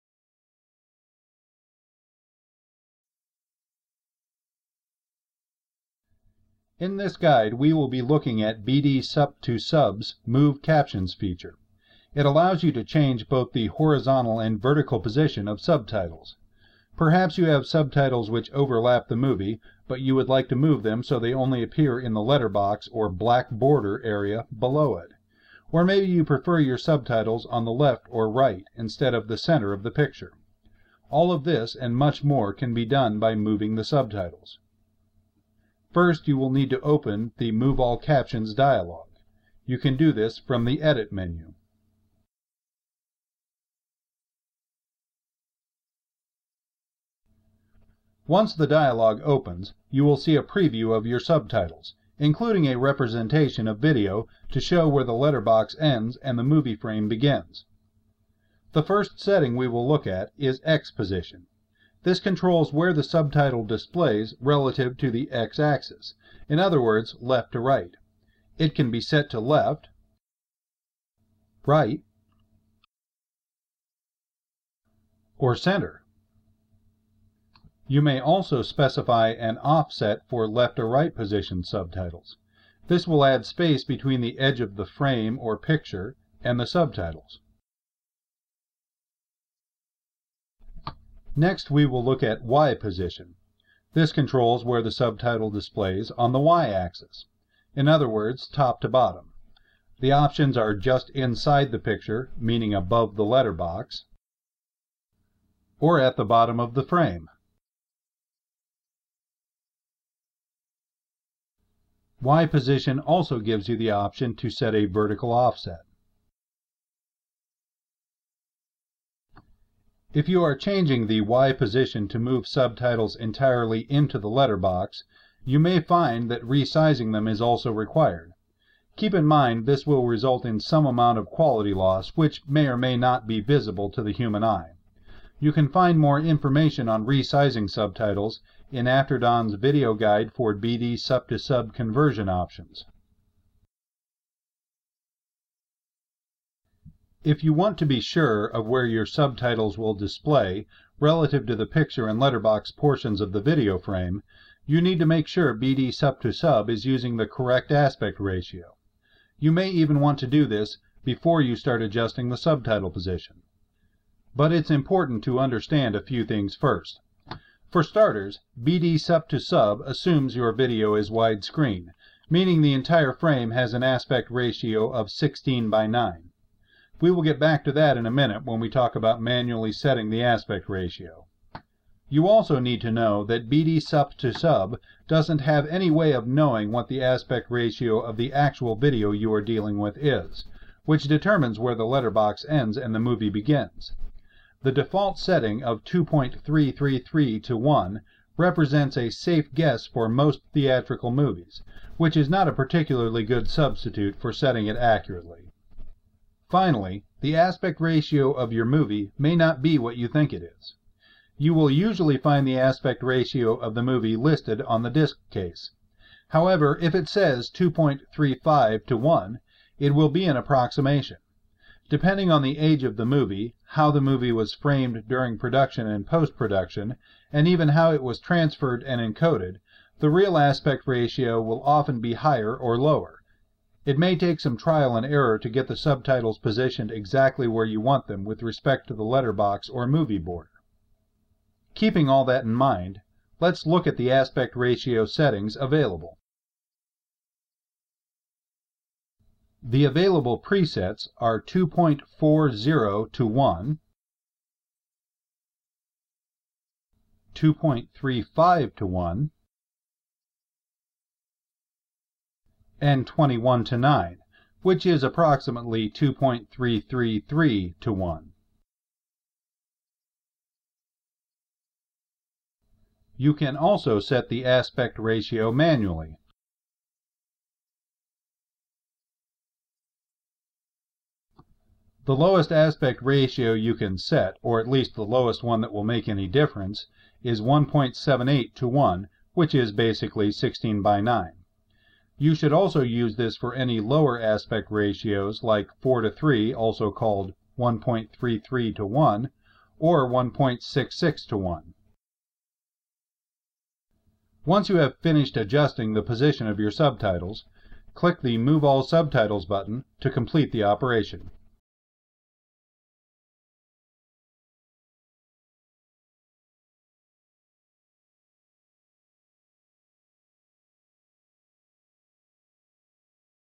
BDSup2Sub - Move Subtitles
A guide to using the Move Captions feature in BDSup2Sub